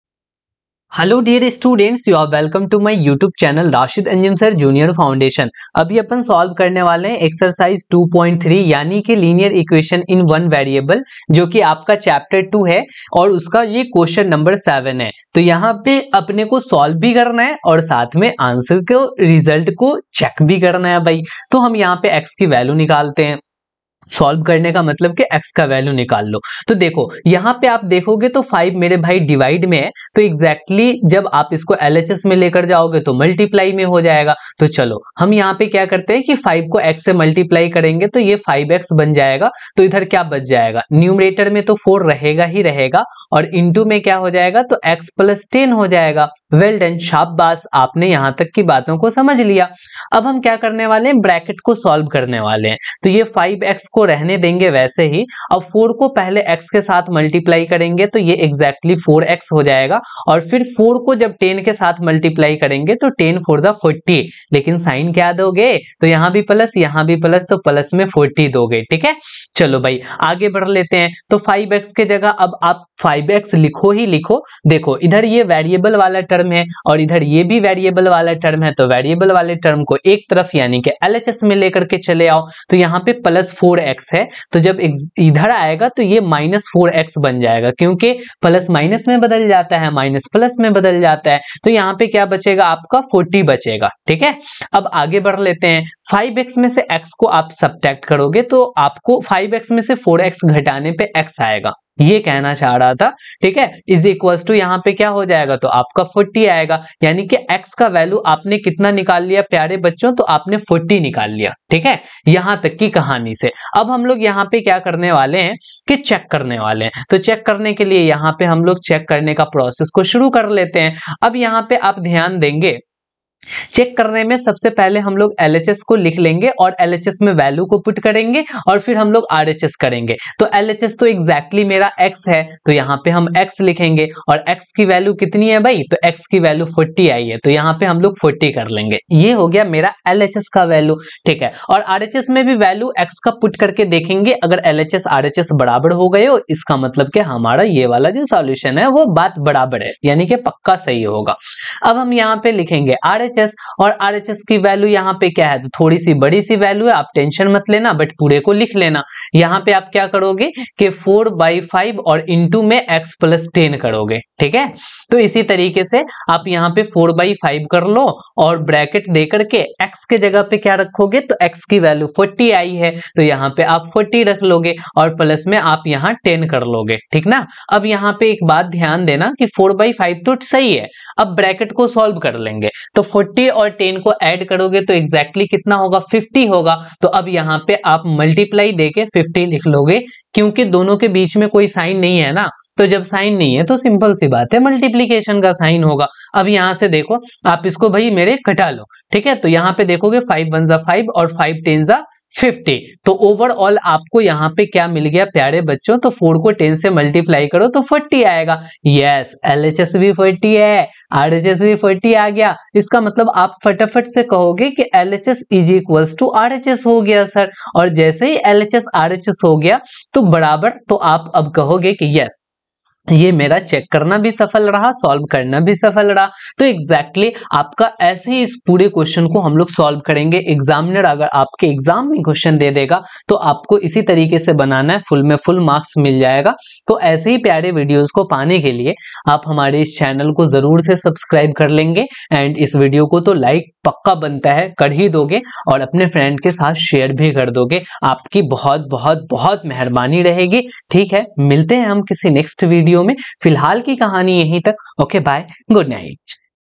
Class 8 | Q7 - Ex 2.3 | Linear Equation in One variable | NCERT Maths | Chapter 2 | CBSE
✸In this video I have discussed the following topics-
◆ Solution Exercise 2.3

UPI Id -
anjumsir@axl

Aapka dil❤️se bhot bhot dhanyewad rhega🙏.

                                                            “Rashid Anjum Sir"